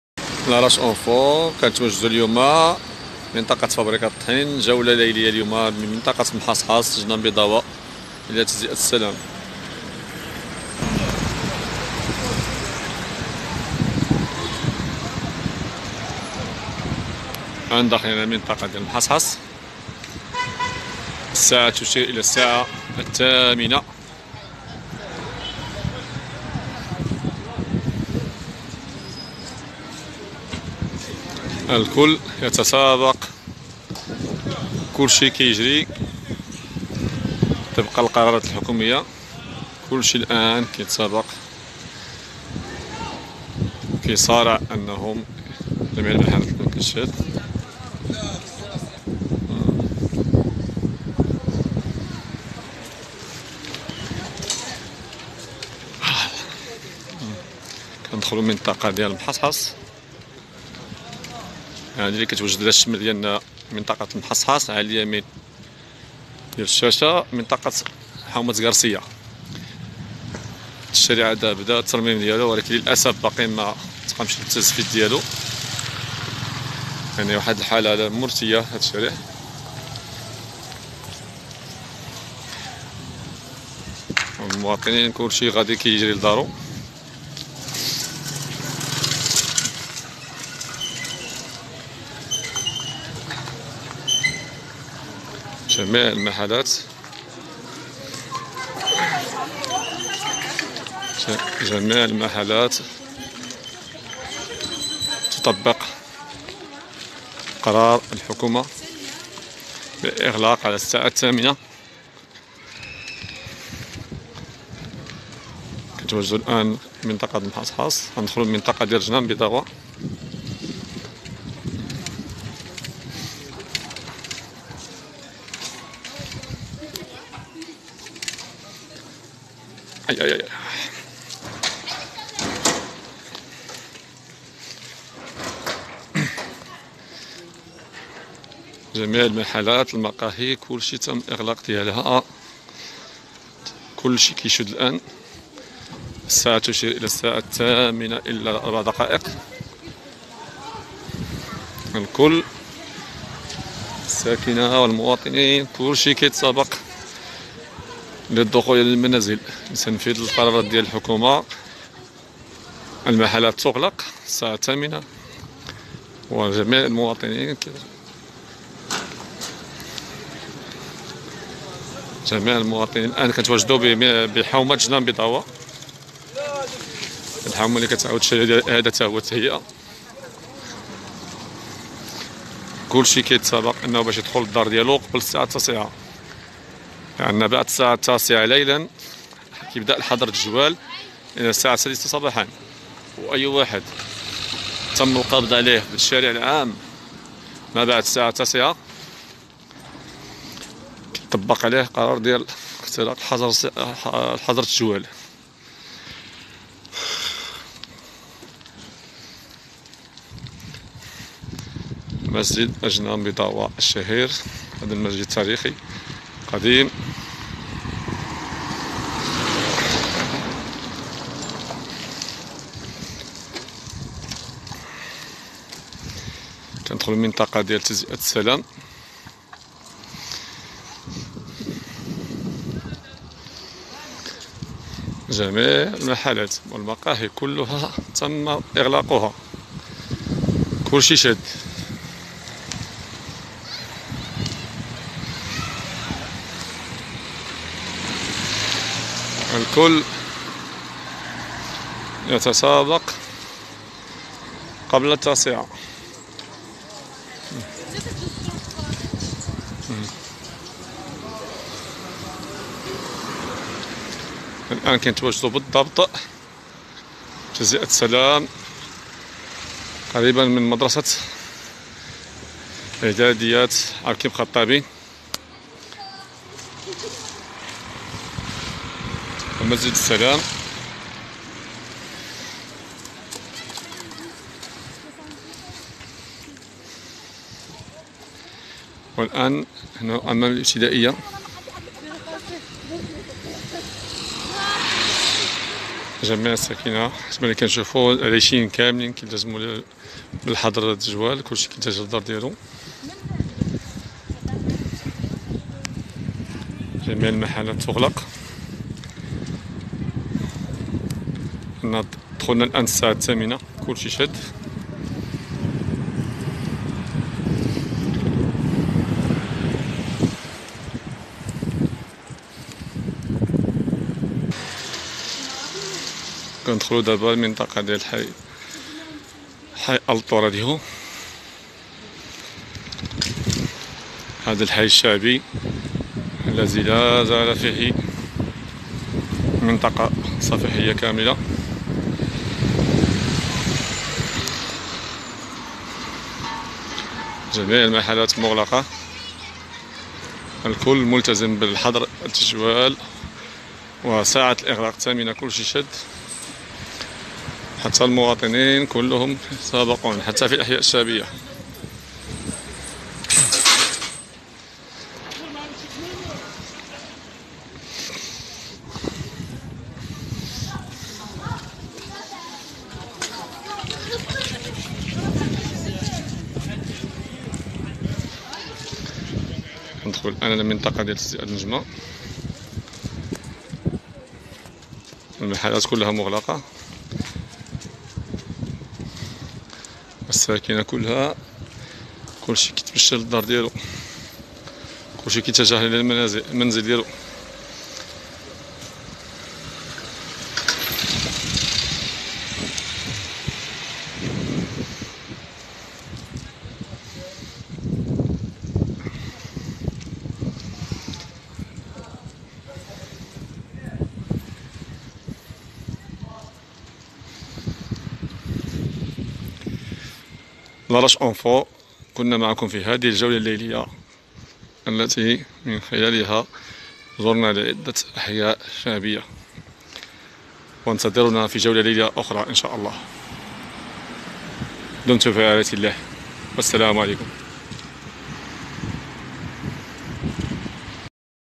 جولة ليلية بحي المحصحص وجنان بيضاوة وحي السلام بالعرائش التزام بتنفيذ قرارالاغلاق
جولة ليلية بحي المحصحص وجنان بيضاوة وحي السلام   بالعرائش التزام الساكنة بتنفيذ شامل لقراراغلاق المحلات و بدأ حظر التجول الليلي لحالة الطوارئ الصحية اليوم السبت 26دجنبر 2020 اليوم الرابع أحضر التجول الليلي.